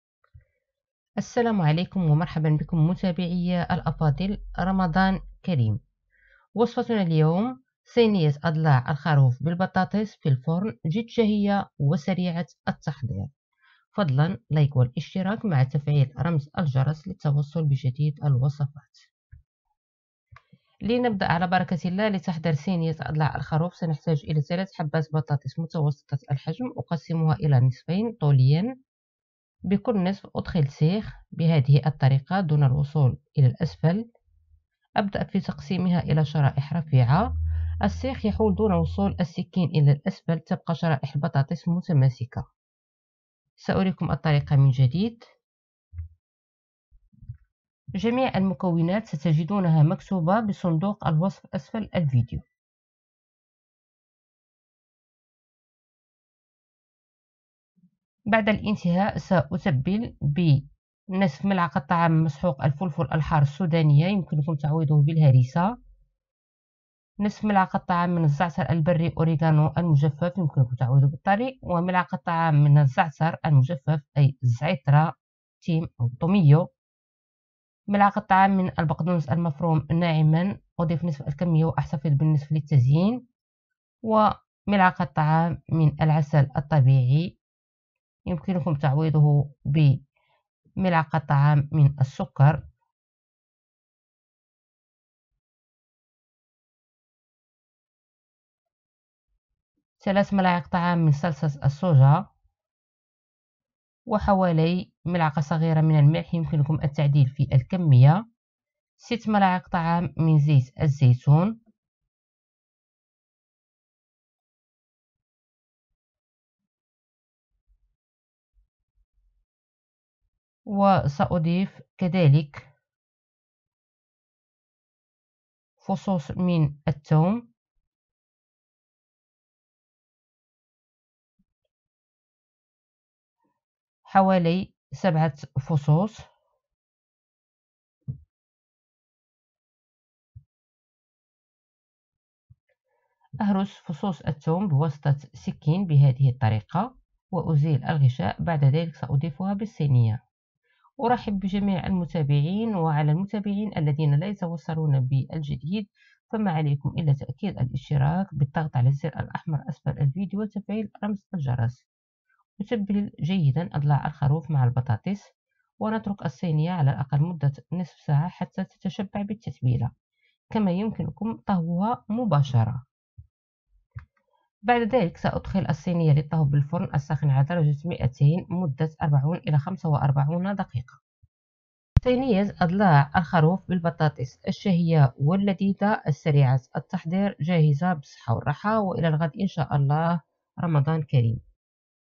وجبة عشاء سهل سريع فطور سحور عشاء رمضان 2019 (2)
من شهيوات رمضان الشهية اقدم لكم عشاء خفيف متكامل صينية اضلاع الخروف بالبطاطا وهي اكلة لذيذة مغدية وصحية وتحضر في دقائق بدون تعب او مجهود تابعوني يوميا في وصفات رمضان مع الشيف نادية
مقادير:
750 غرام اضلاع خروف
3 حبات بطاطس
نصف ملعقة طعام مسحوق الفلفل الحار الهريسا او السودانية
نصف ملعقة طعام زعتر بري مجفف اوريغانو
ملعقة طعام زعتر او زعيترة
1 ملعقة طعام بقدونس مفروم
1 ملعقة طعام عسل طبيعي أو سكر
3 ملاعق طعام صوص الصوجا
1 ملعقة صغيرة ملح
6 ملاعق طعام زيت نباتية
7 فصوص ثوم

شاهد ايضا:
قناة الطبخ تقدم وصفات ناجحة ومبسطة من المطبخ المغربي العربي والعالمي

اشراف عام
الحسين ساخي